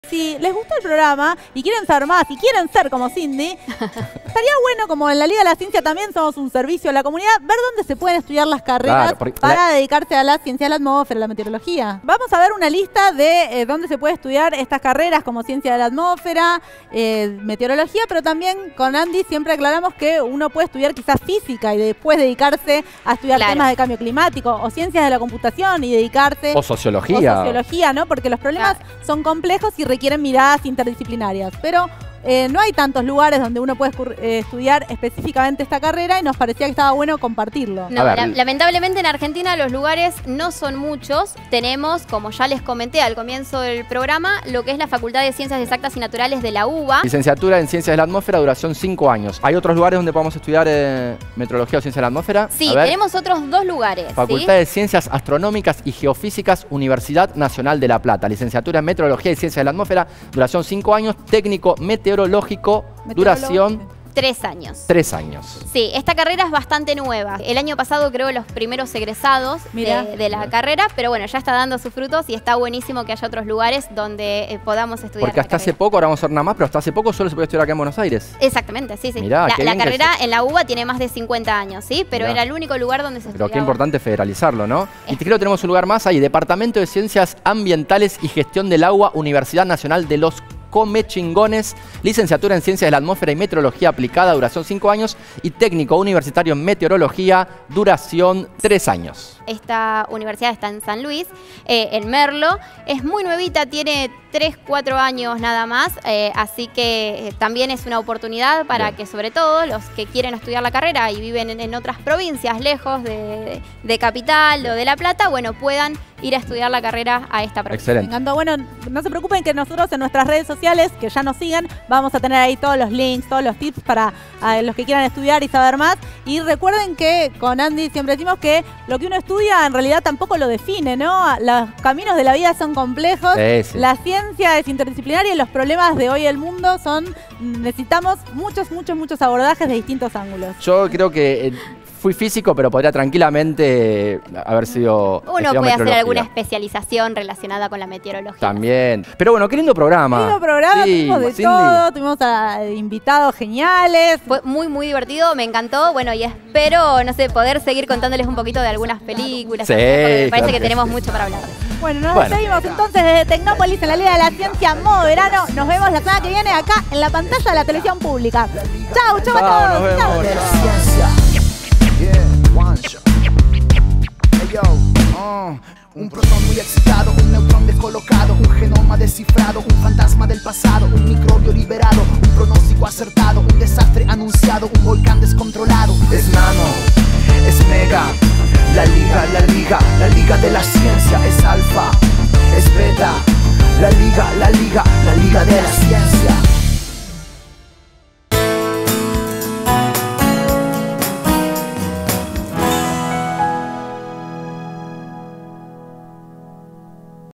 Despertar vocaciones: Dónde estudiar Meteorología y Ciencias de la Atmósfera?
Preparamos información para los que están buscando carreras científicas.
Programa emitido el 26/02/22